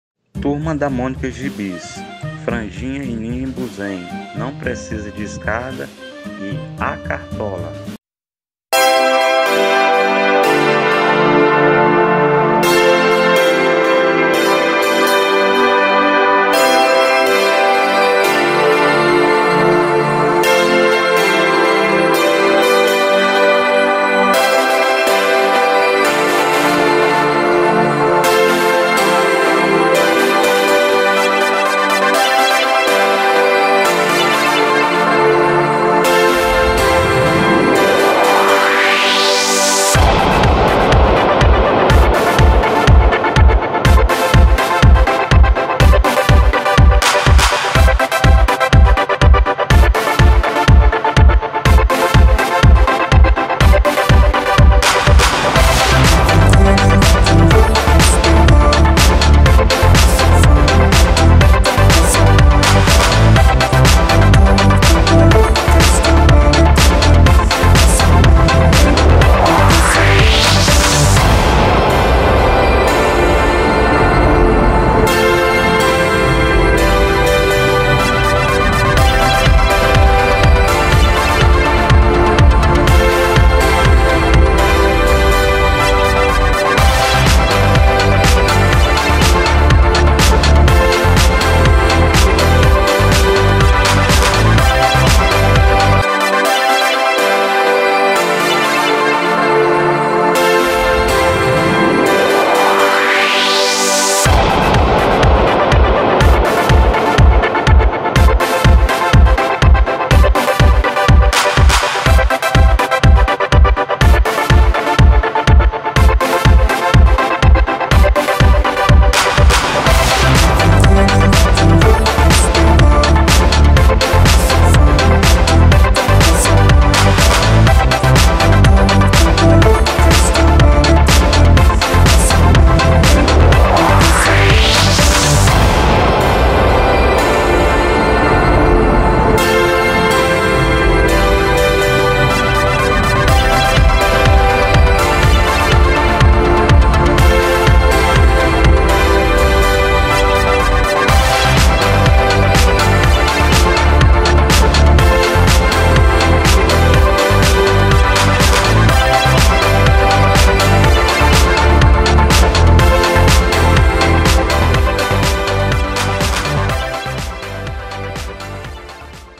Franjinha e Nimbus em Não precisa de escada e A cartola – Gibi da Turma da Mônica
Franjinha e Nimbus em duas historinhas – assista – Franjinha e Nimbus em Não precisa de escada e A cartola – Gibi da Turma da Mônica